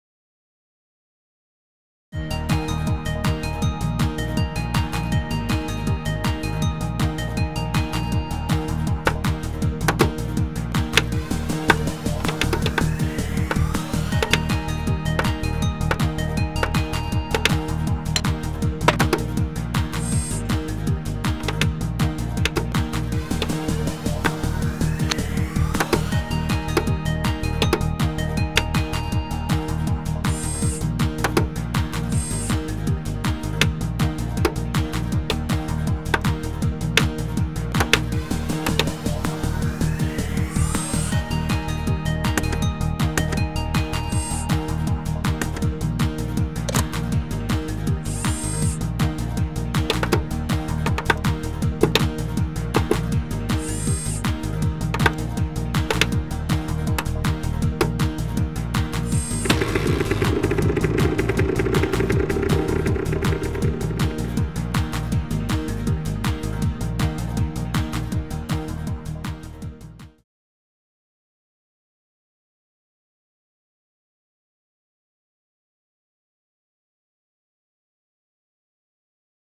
Lego classic elements Scooter formation | Brick builder | Lego learn | Kiddiestv
Learn how to build a LEGO scooter from LEGO bricks and LEGO classic elements | LEGO Scooter fun and learn | Brick builder | Youtube LEGO | Youtube kids